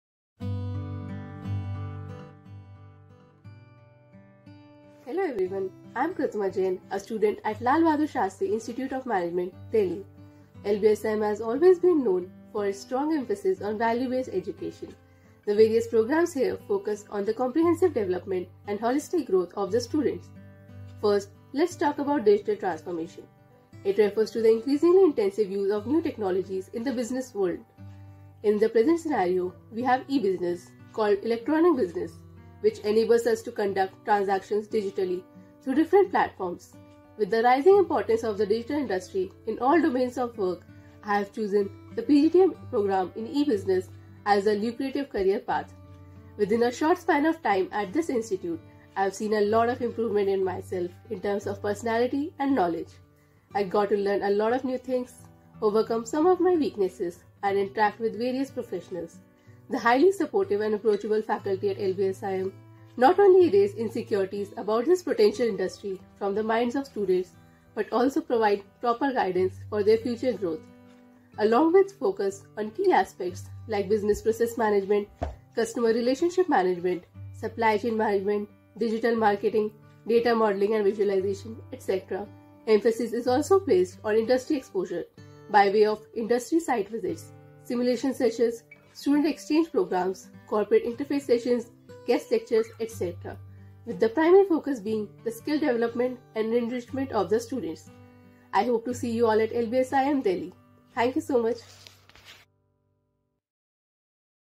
LBSIM PGDM E Business Experience | Lal Bahadur Shastri Institute of Management
Elevate your career with our PGDM E-Business program, a new-generation industry-oriented program.

Learn the latest technologies and trends such as Cloud Computing, Internet of Things, E-CRM, Digital Transformation, Blockchain Tech, FinTech, and Big Data Analytics to stay ahead in the digital economy.

Apply now for 2 years PGDM Programme and take the first step towards a successful career in the digital era.